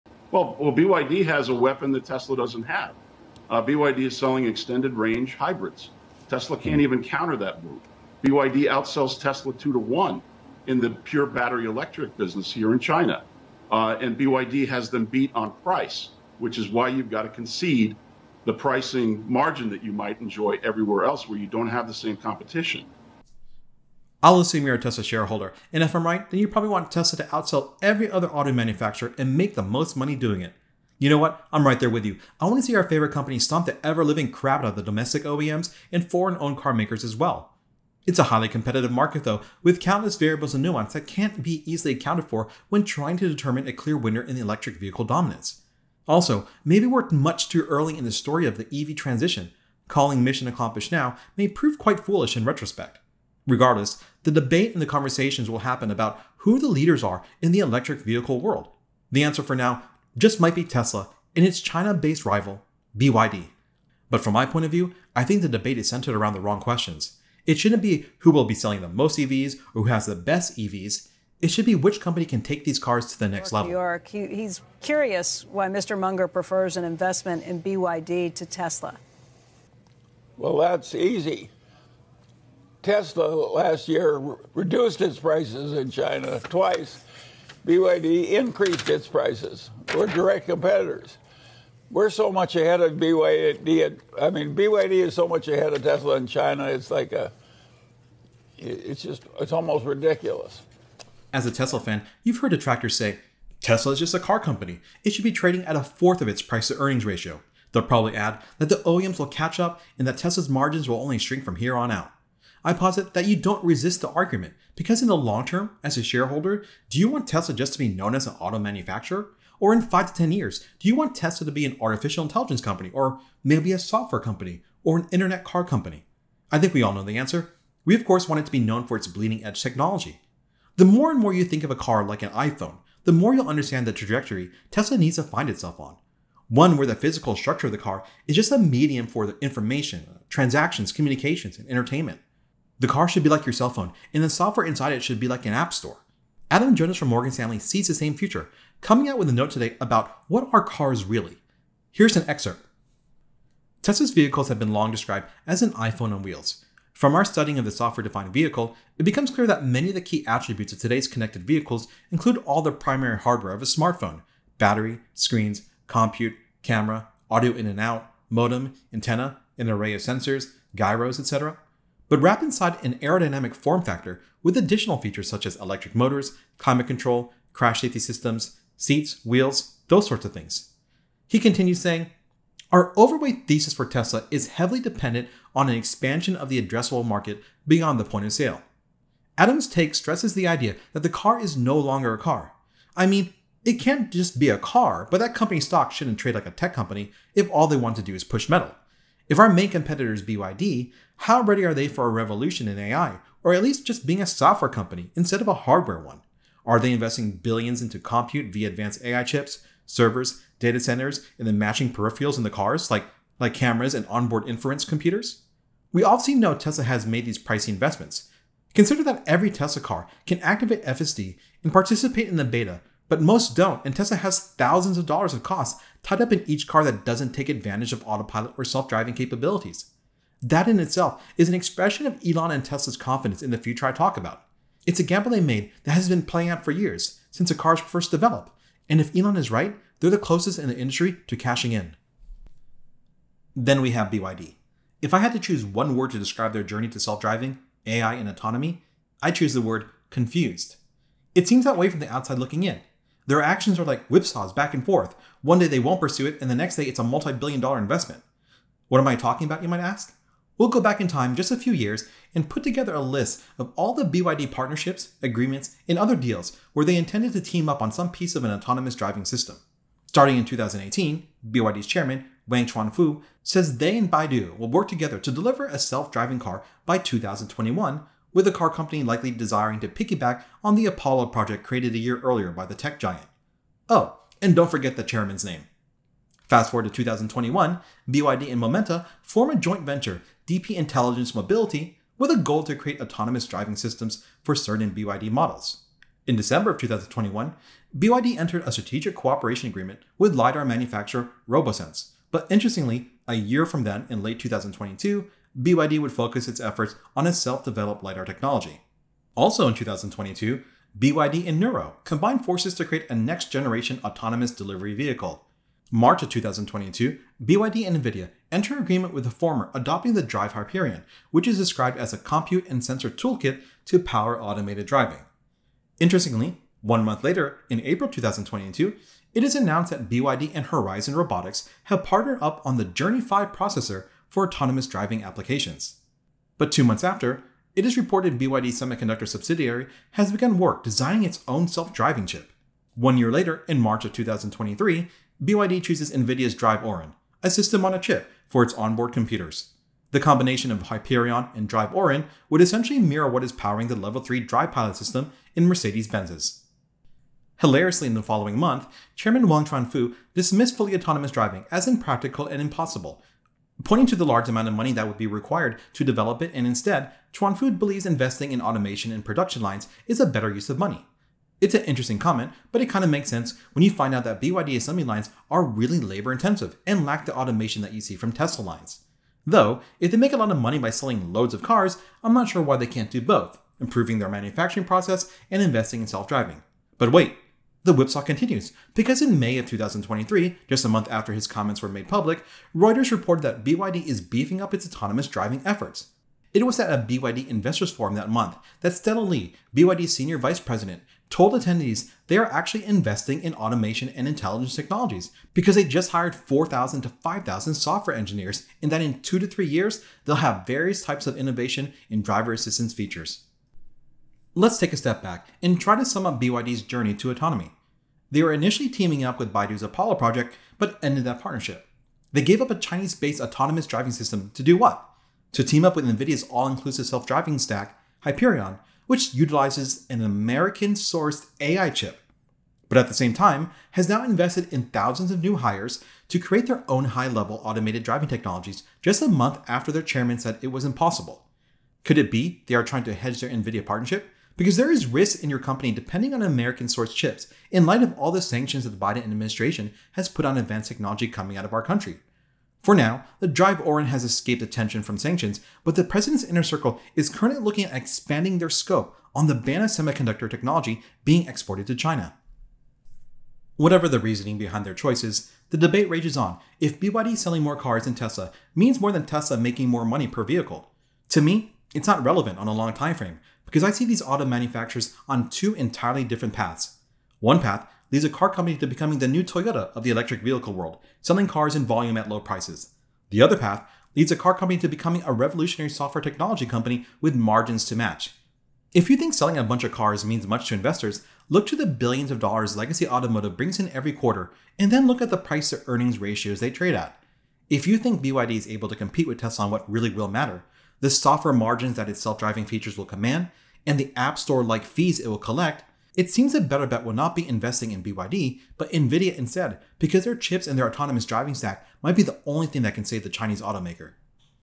Tesla vs. BYD Isn't a Competition Because the Chinese Automaker is Falling Behind in Autonomy and AI
Tesla vs. BYD. A story as old as time, or at least 3 years old, and it's not one about Chinese government subsidies or hybrid cars being unfairly counted towards EVs. Who will come out on top then? Some say Tesla is winning will only hang onto the sales lead for a few months. I pose a different criteria for determining the winner of the EV crown instead of total BEV sales. It should be based on who transcends the automotive world to high margin software and technology.